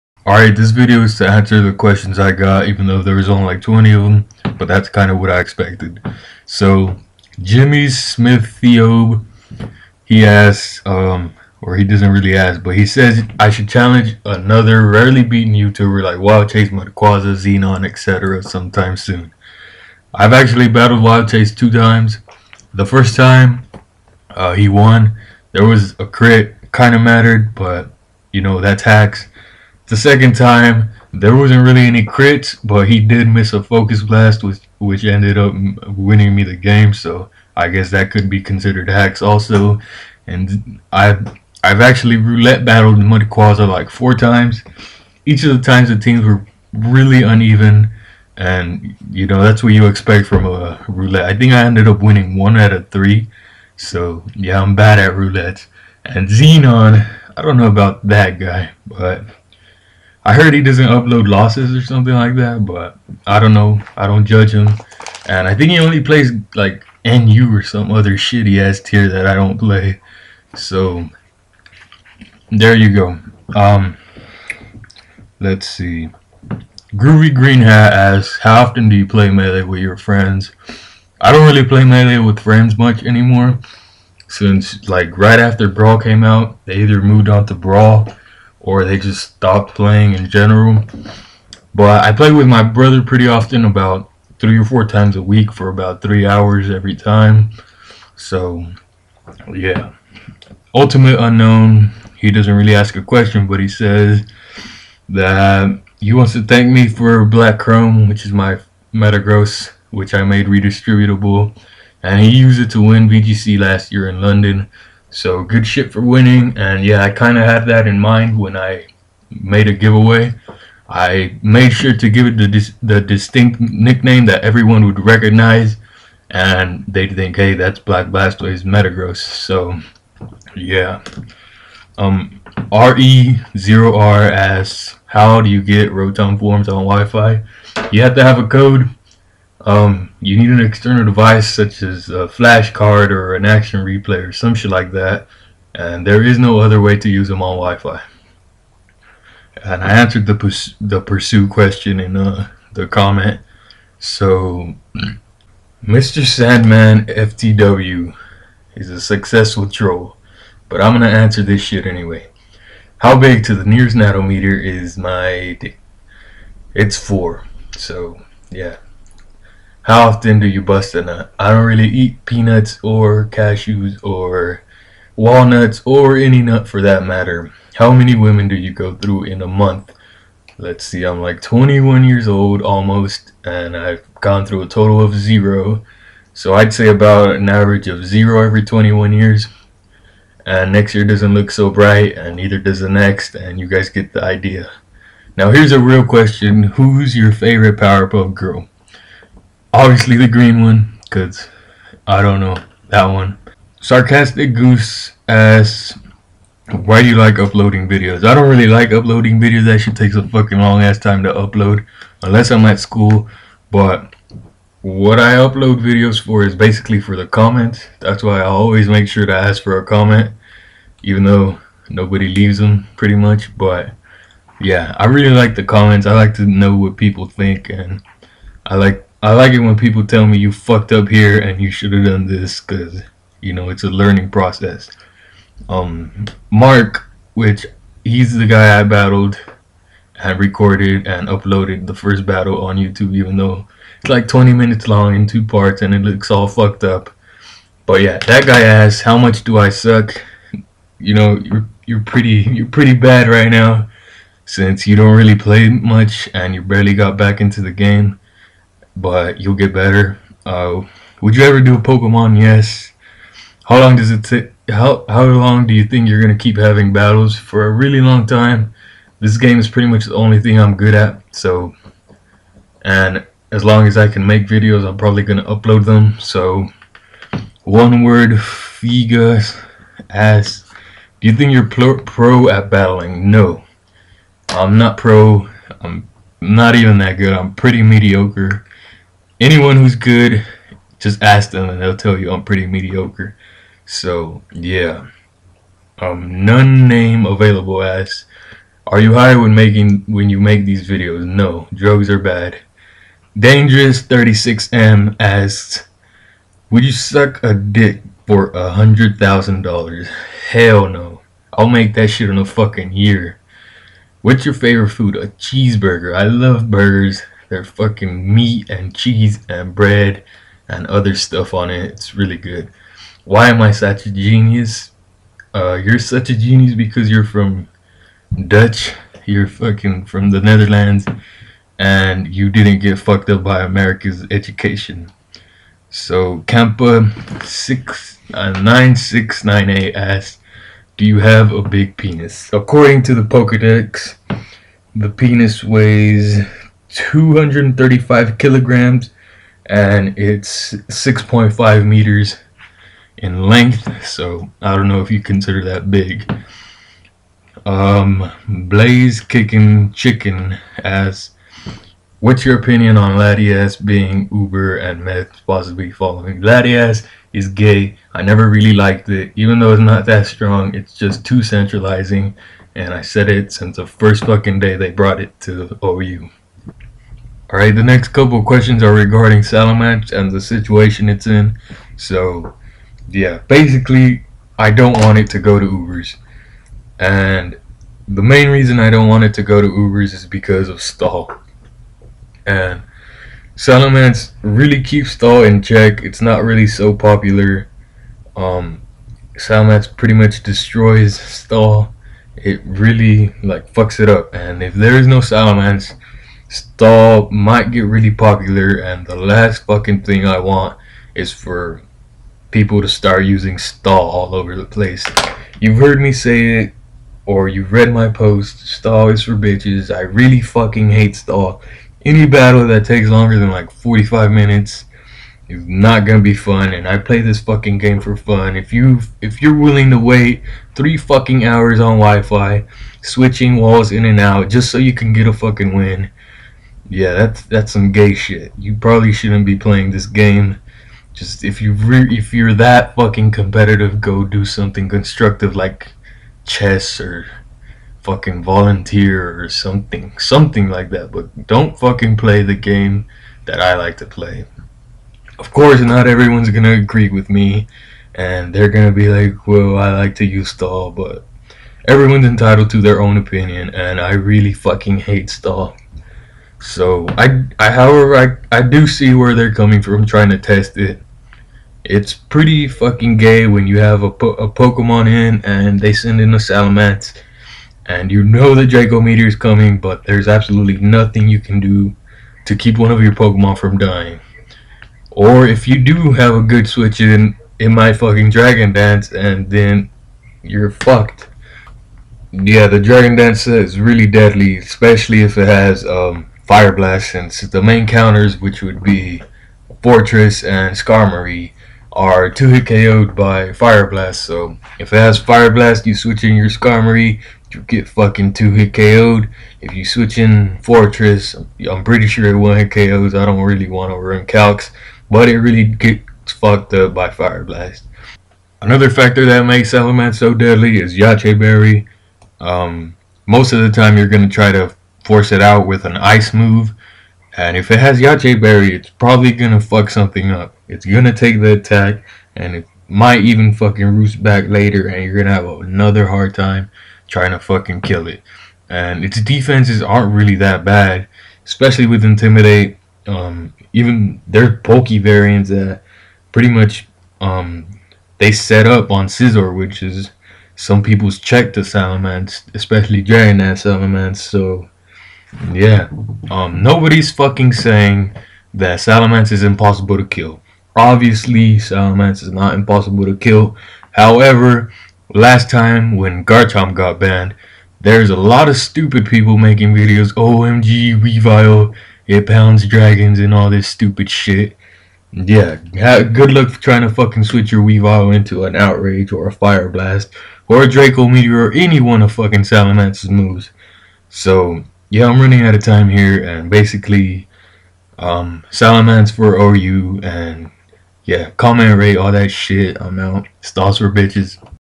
Answer video + Random Battle
Here's the answers to the questions I got. I could have said much more about Salamence but I ran out of time and I don't want to make this thing two parts.